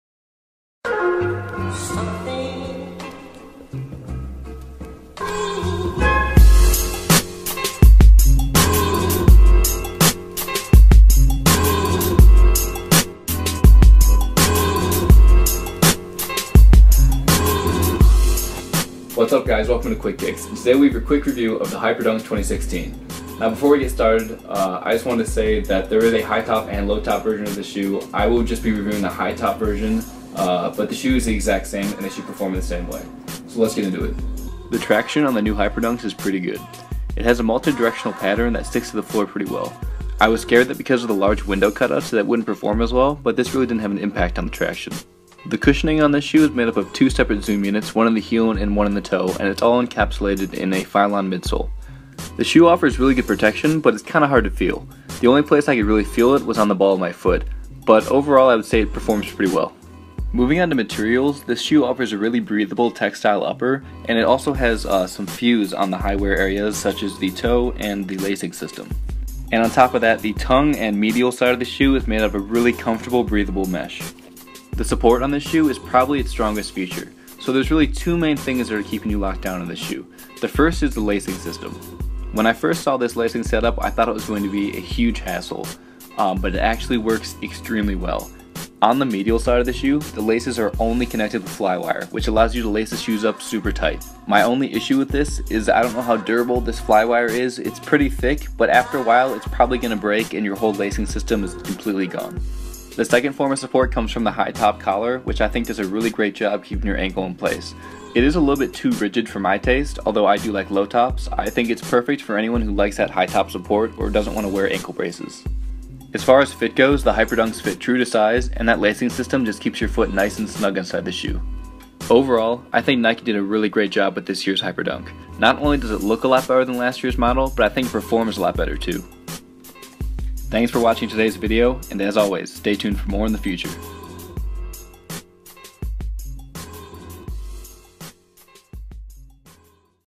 Nike Hyperdunk 2016 Review
Welcome to our review of the new Nike Hyperdunk 2016!  I really think Nike did a great job with the Hyperdunk line this year in both the design and performance aspects of the shoe!  Let us know what you think and if you're going to pick some up in the description.